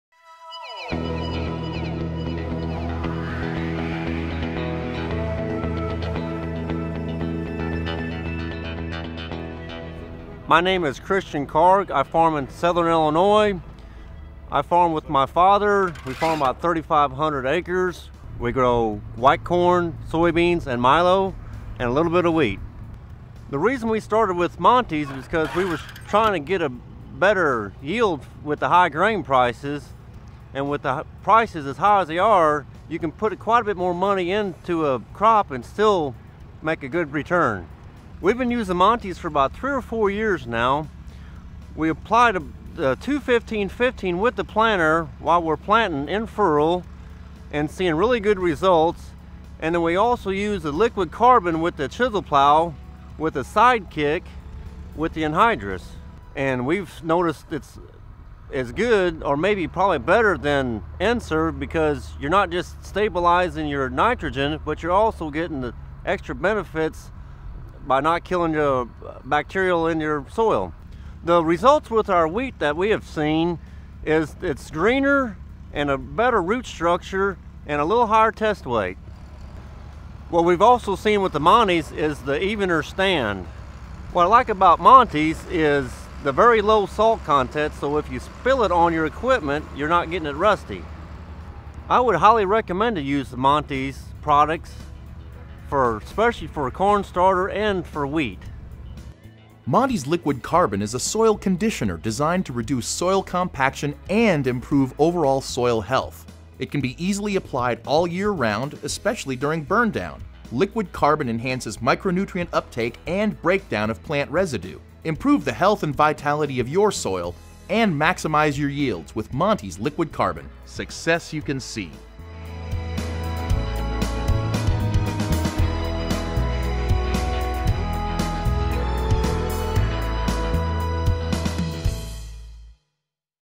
Monty's Liquid Carbon on corn (Christian Carg)
Farmer Christian Carg talks about Monty's Liquid Carbon on corn